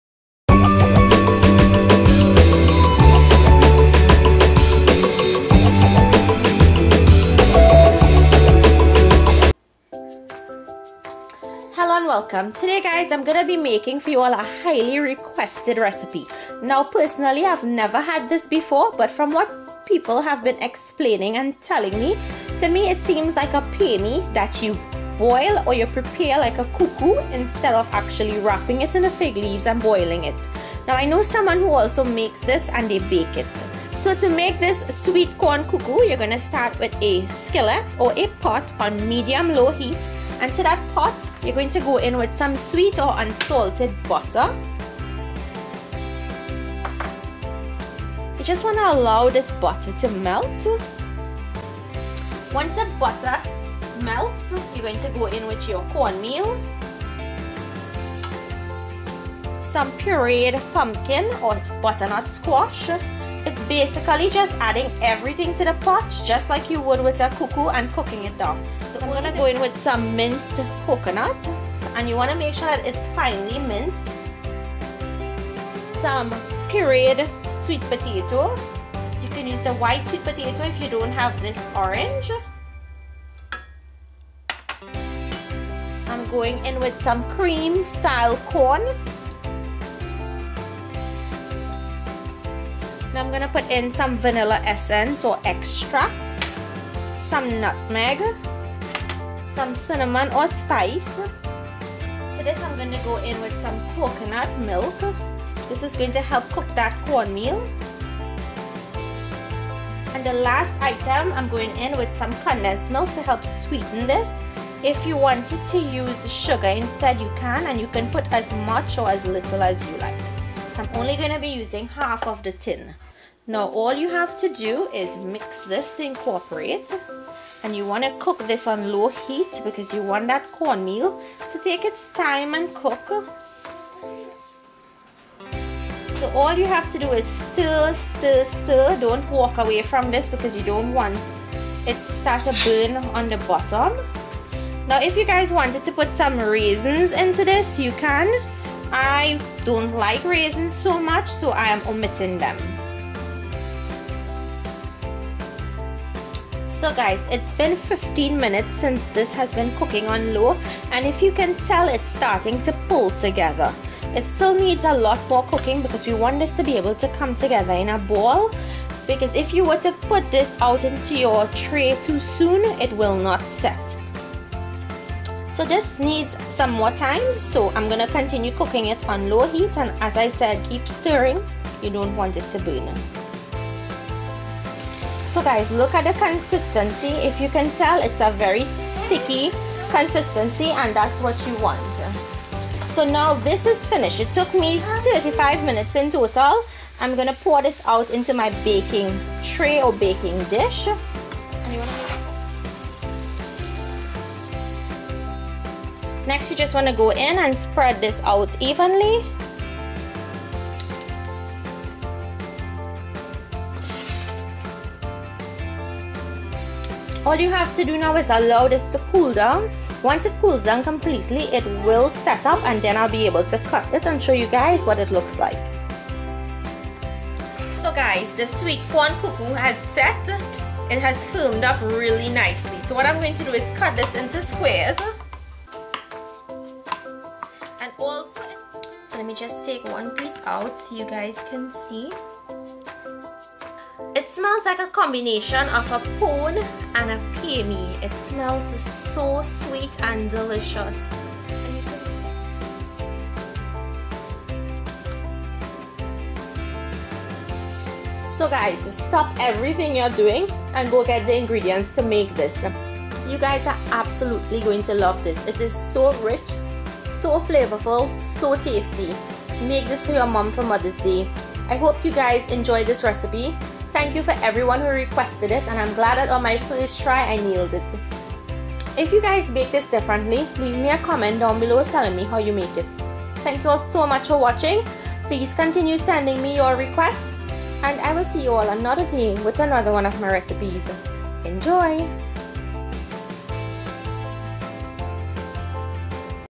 Sweet Corn Coo Coo | Sweet Cou Cou - Mother's Day Recipe - Episode 591
2 cups yellow fine corn meal
2 cups coconut milk
6 tbsp unsalted butter
1/2 tin condensed milk
1 cup finely grated coconut
1 cup pureed pumpkin
1/3 cup pureed sweet potato
1 tsp nutmeg
1 tsp cinnamon
1 tsp vanilla essence or mixed essence
1/2 cup cream corn

OR
send product to